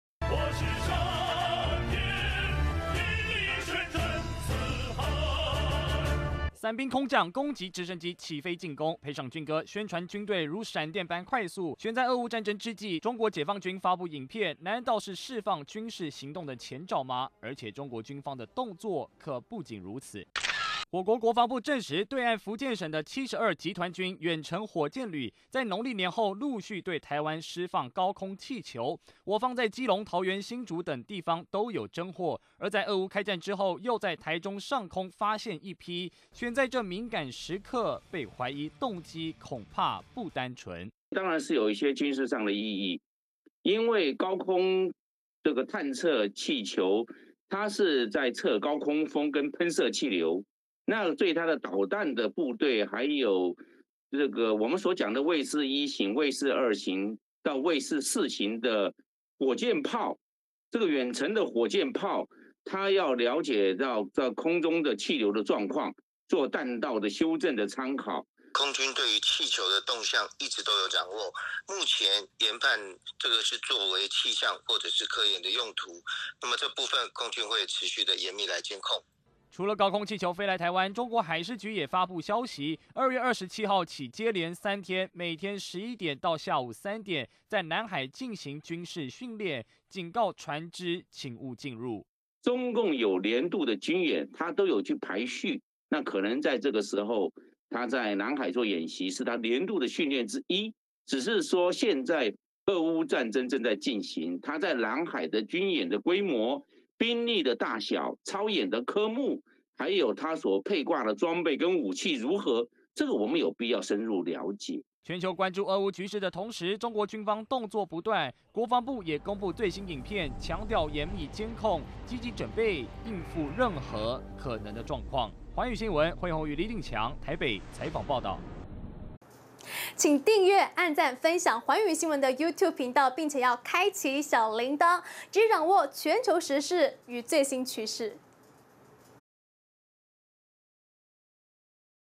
共軍高空氣球飄台 退將判斷可能目的@globalnewstw
中國最近的軍事動作不斷，國防部證實，有解放軍施放的高空氣球，在俄烏戰爭之際，繼續飄到台灣上空，前空軍副司令張延廷分析，這絕對有軍事意涵，因為中國可以蒐集台灣上空的噴射氣流資料，有利於解放軍的導彈攻擊，對此，國君強調會嚴密監控，做好萬全準備。